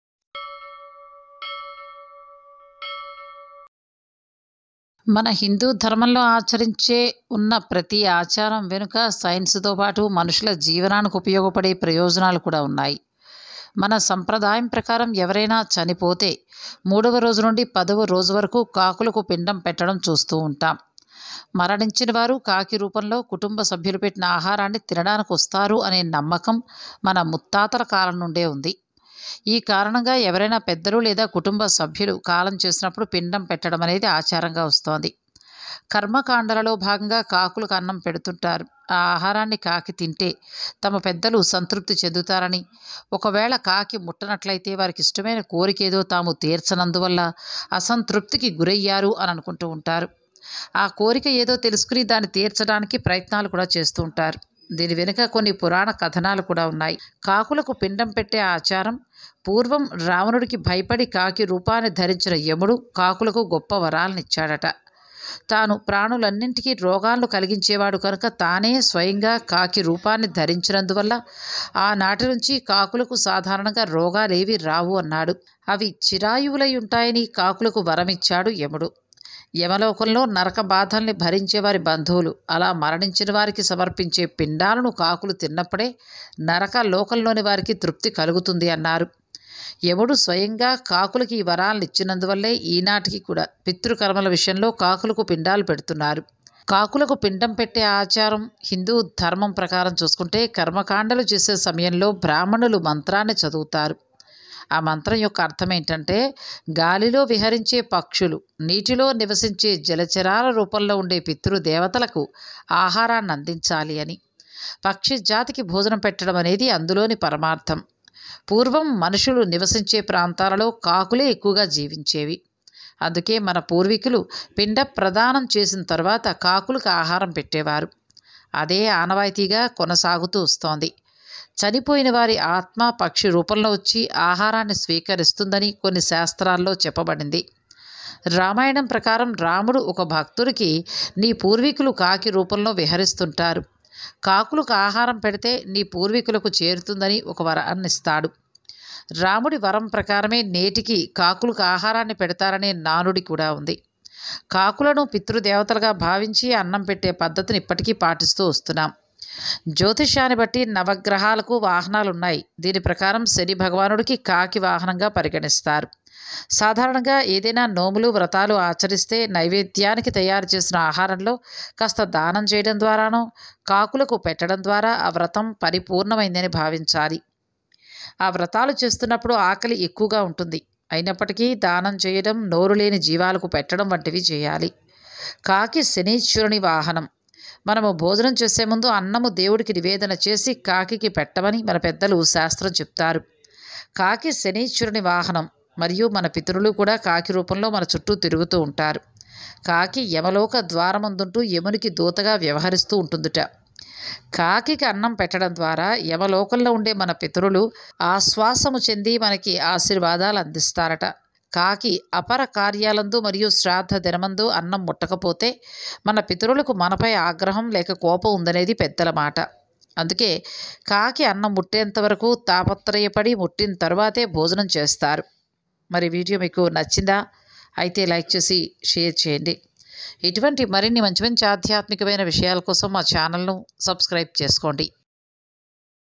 రోజు కాకికి అన్నం పెడితే మీకు జరిగేది ఇదే!! Feeding Crows Astrology | Significance of Feeding CROWS
రోజు కాకికి అన్నం పెడితే మీకు జరిగేది ఇదే!! Feeding Crows Astrology | Significance of Feeding CROWS | Why should we feed Crows | Benefits of Feeding Birds Astrology | Bhakthi Samacharam